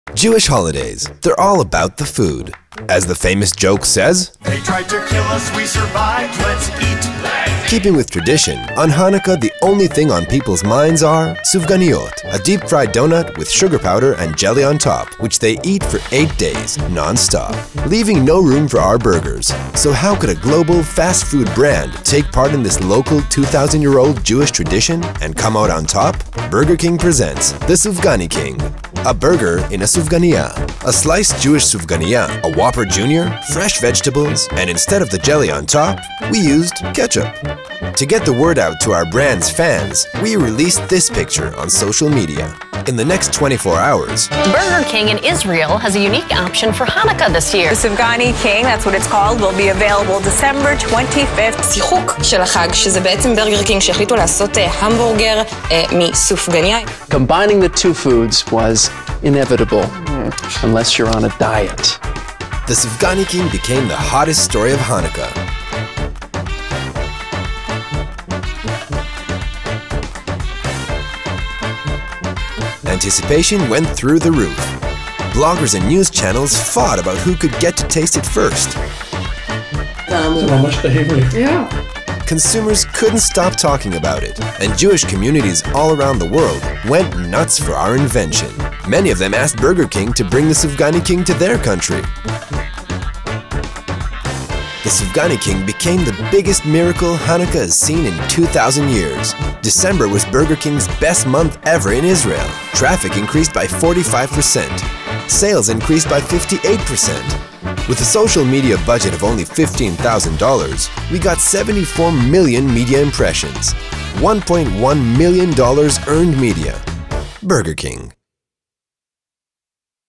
Burger King "SufganiKING"
This Hanukkah promotion by Burger King generated international headlines.,The global burger chain launched a limited edition treat specifically for the Israeli market that leveraged the insight that Israelis love eating traditional Sufganiyot doughnuts during Hanukkah. The new BK SufganiKing is a burger made with these traditional doughbuts, and this new menu item proved to be a hit during Hanukkah.